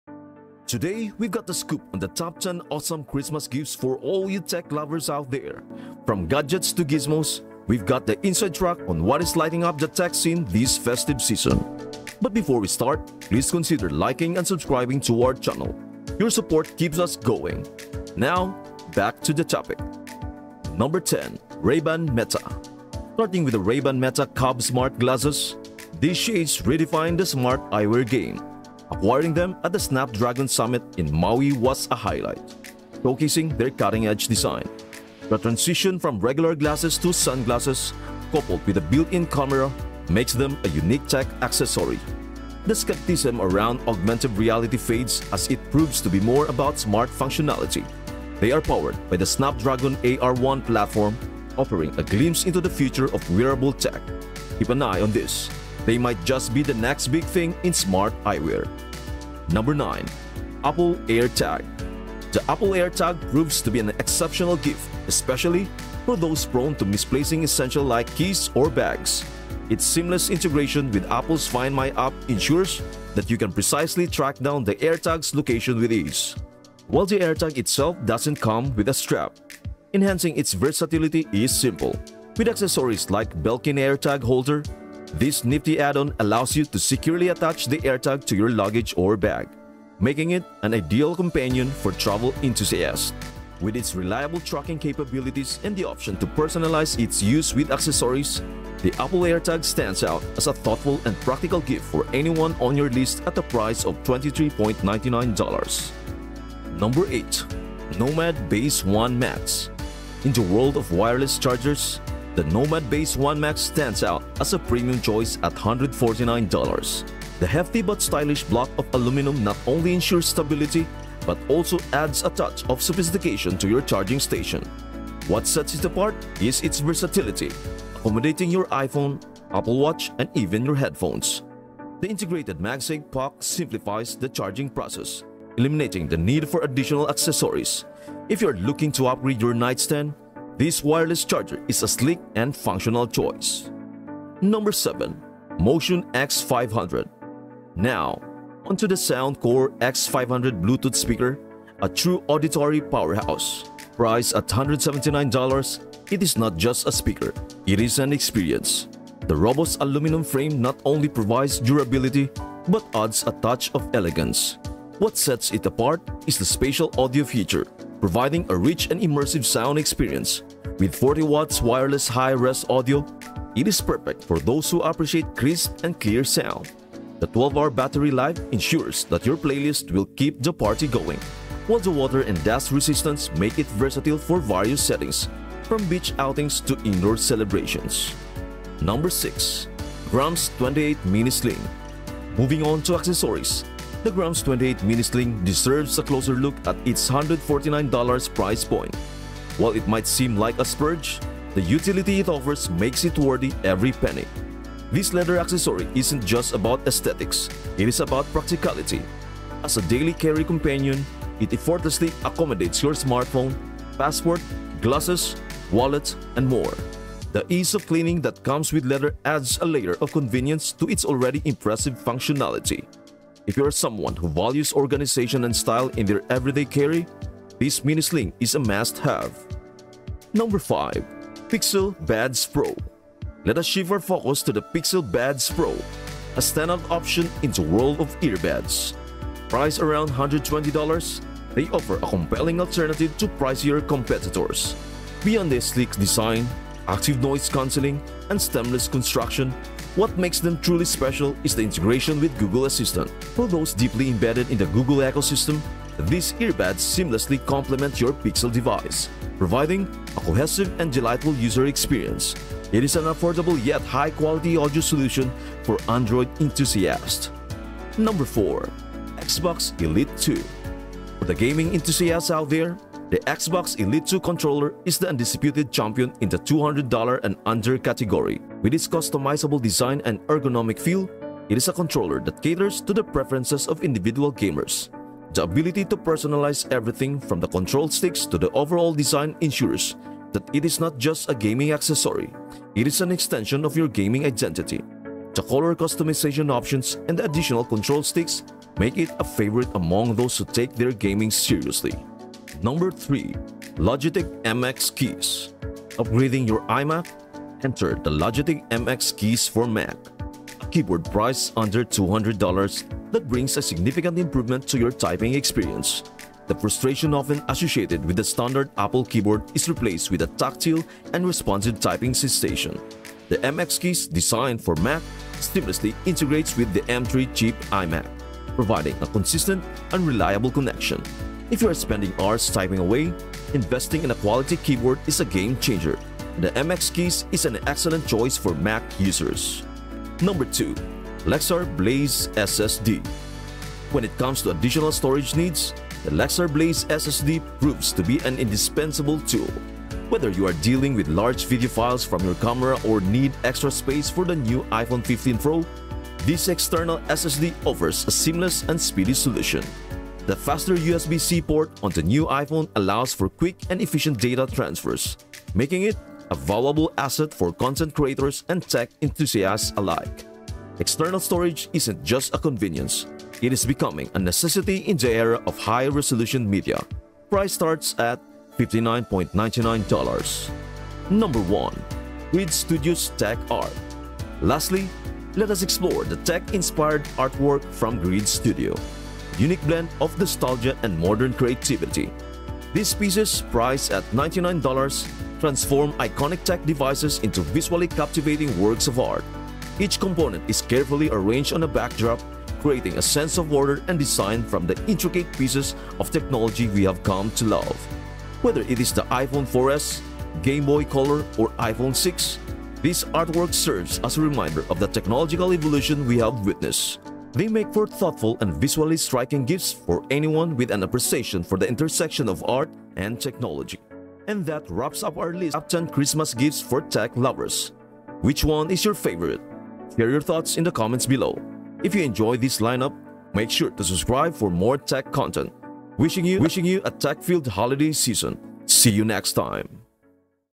Top 10 Must-Have Christmas Tech Gifts for 2024: Perfect for Tech Lovers!
In this video, we present the top 10 must-have Christmas tech gifts for 2024. From cutting-edge gadgets and innovative devices to the latest in smart home technology, these holiday tech gifts are perfect for any tech enthusiast. Watch now to find the ideal present for the tech lover in your life and make this holiday season unforgettable!

Timestamps:
0:00 - Intro
0:21 - Ray Ban Meta
1:00 - Apple Air Tag
1:47 - Nomad Base One Max
2:25 - Motion X500
3:09 - Grams 28 Mini Sling
3:51 - Pixel Buds Pro
4:28 - Xbox Elite 2
5:03 - Logitech MX Keys
5:40 - Lexar Blaze SSD
6:21 - Grid Studios Tech Art
7:04 - Outro

"Looking for the perfect gift for a tech enthusiast? Share your favorite tech gift idea in the comments below!"